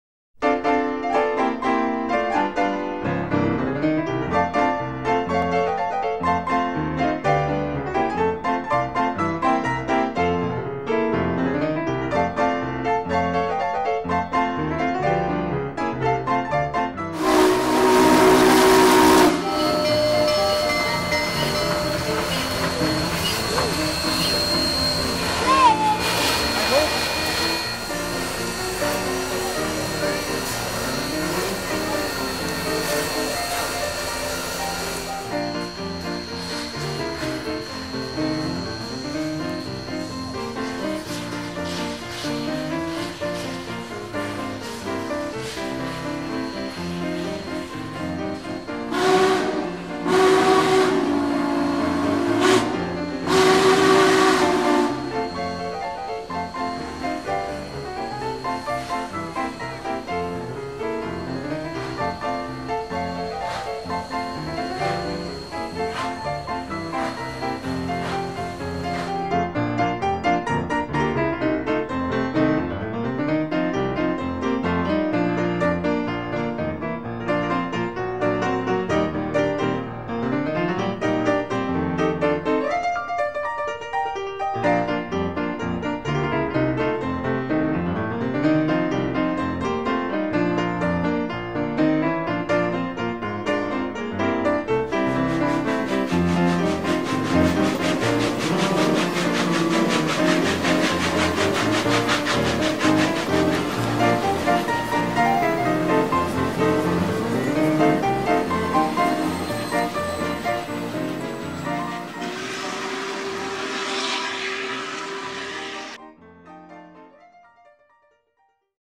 Steam Train Stops in Washington
On Thursday, June 2, Union Pacific's steam locomotive No. 844 made a stop in Washington, Missouri.